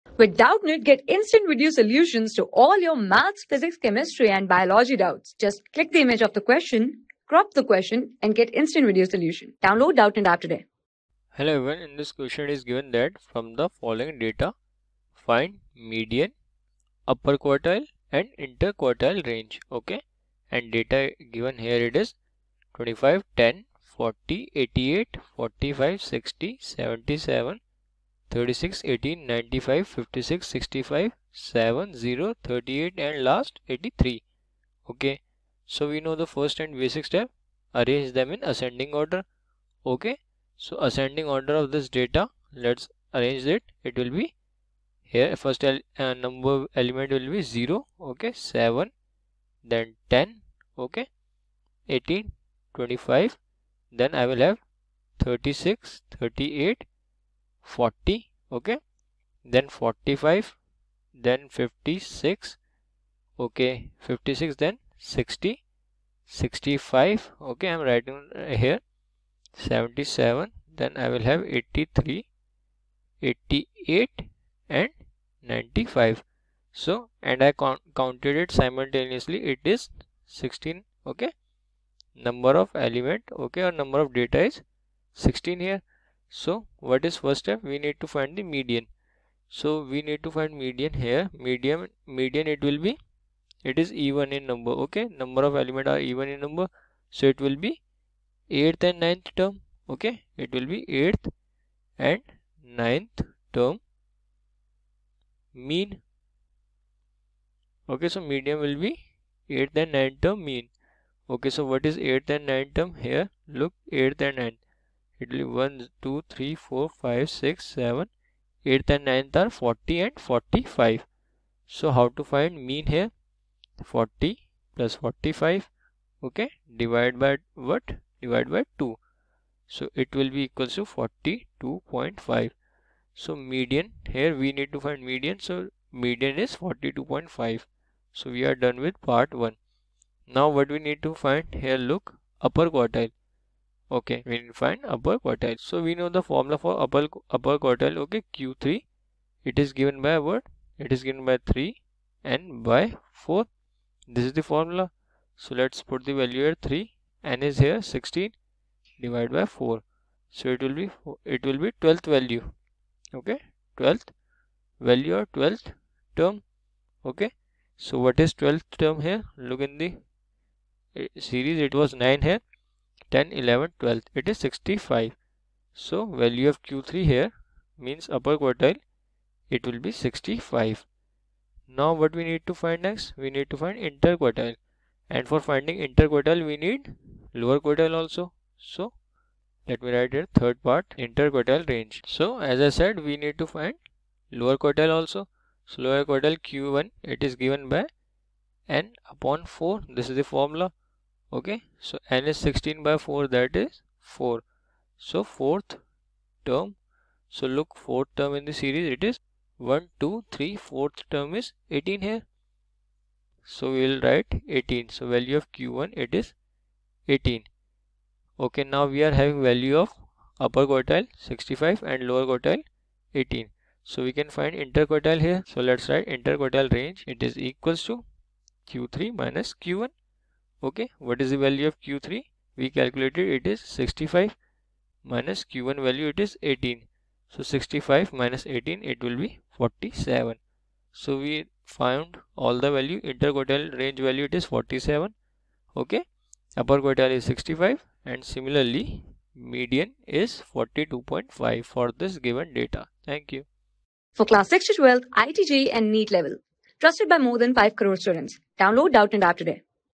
From the following data: find (i) Median (ii) Upper quartile (iii) Inter quartile range: 25, 10,...
From the following data: find (i) Median (ii) Upper quartile (iii) Inter quartile range: 25, 10,40,88,45,60,77,36,18,95,56,65,7,0,38 and 83
Class: 10
Subject: MATHS
Chapter: MEASURES OF CENTRAL TENDENCY (MEAN, MEDIAN, QUARTILES AND MODE)
Board:ICSE

👉 Have Any Query? Ask Us.